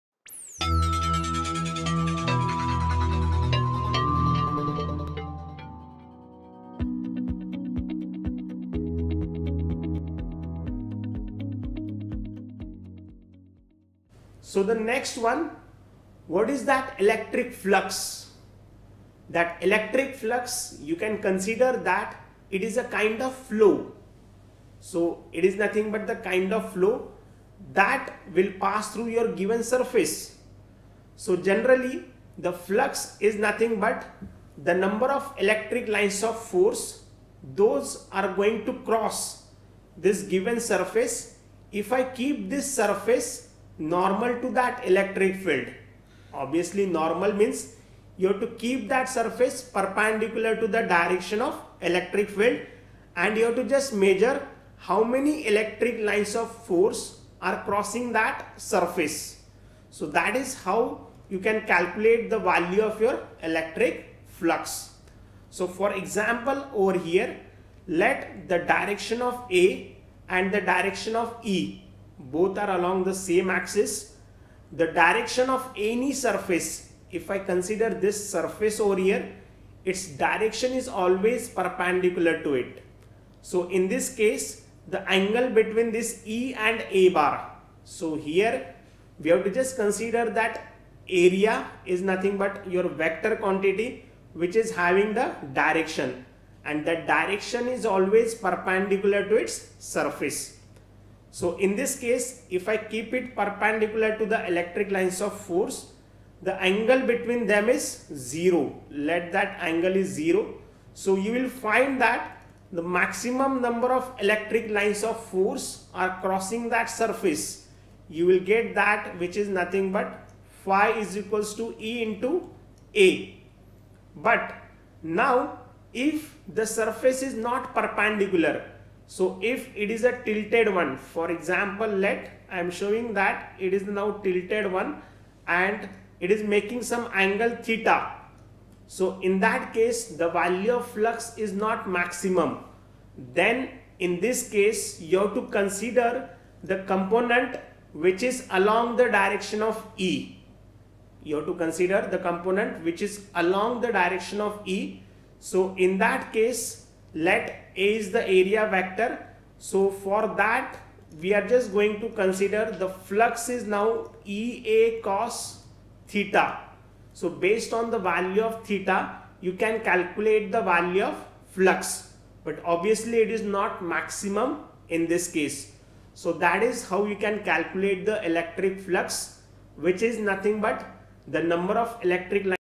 Electric Flux | Electric Field and Charges | JEE | NEET | Physics - TG Campus
In this video, you will learn that electric flux through an area is defined as the electric field multiplied by the area of the surface projected in a plane perpendicular to the field. Learn more about Electric Field and Charges with Tomorrow’s Genius. Let us make learning fun & create Tomorrow's Geniuses today with the best Faculties.